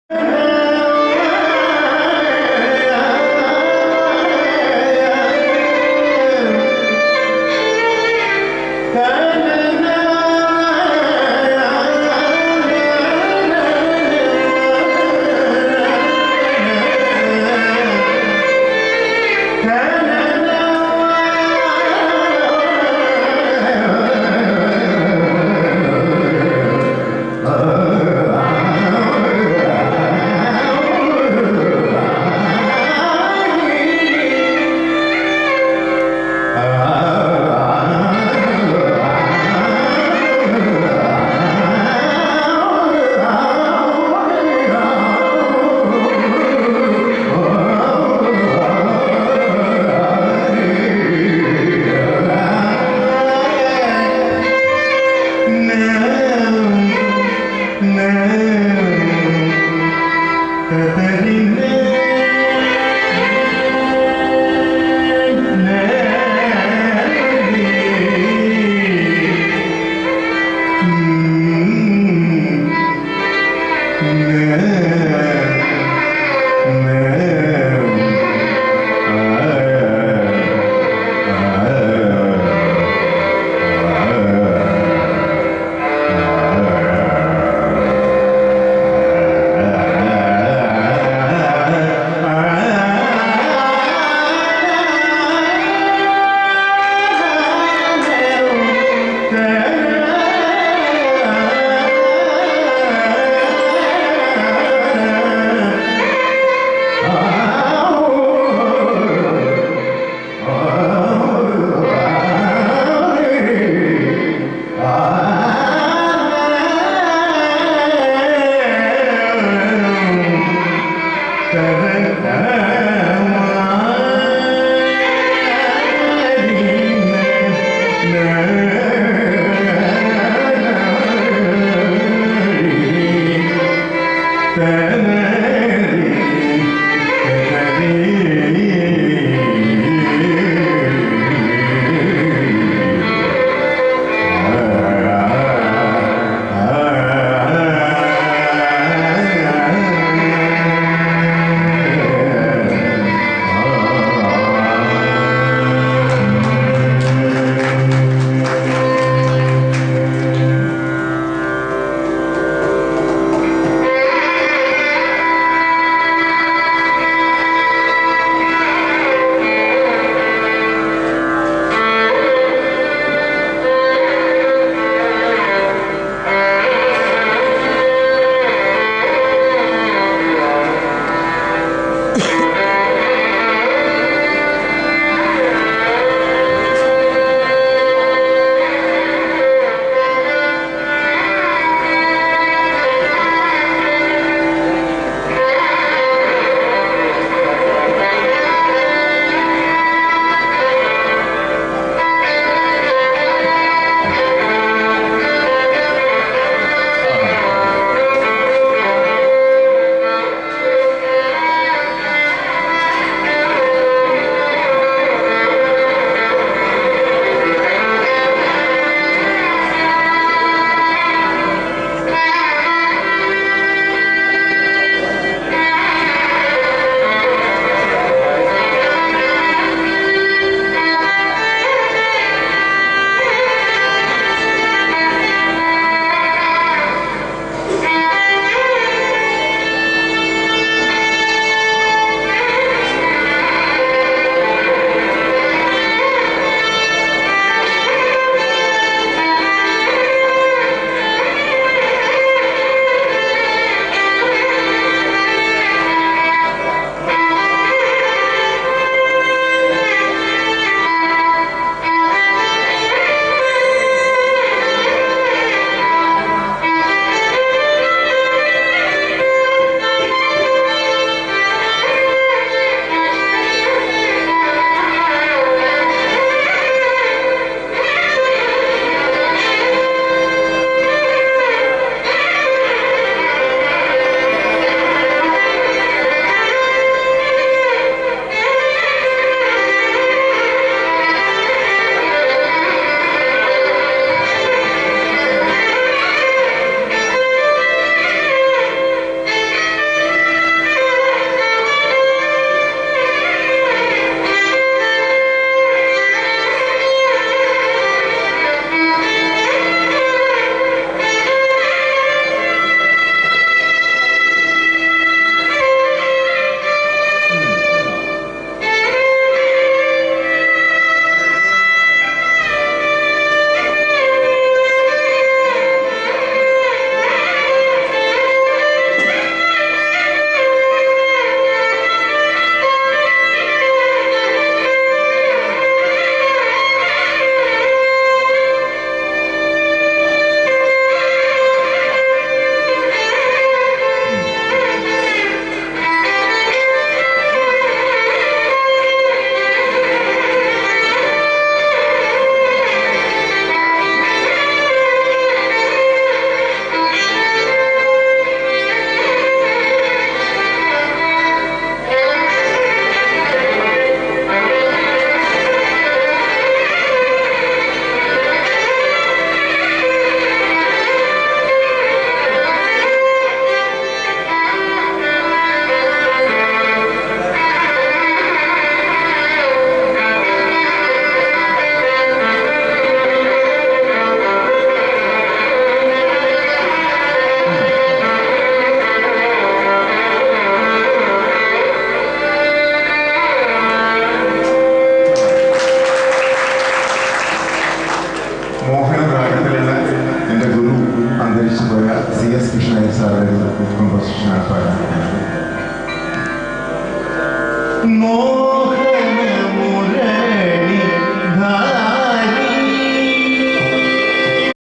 jugal bandhi.harikumar sadanam,kottakkal madhu at palakkad
ജുഗല്‍ബന്ദി,സദനം ഹരികുമാര്‍ ,കോട്ടക്കല്‍ മധു,രാം മോഹന്‍.16/9/2013 at കല്ലെകുളങ്ങര ,പാലക്കാട്‌ [ഓണാഘോഷം ]
അവതരണം ശിവരഞ്ജിനി കലാ സമിതി .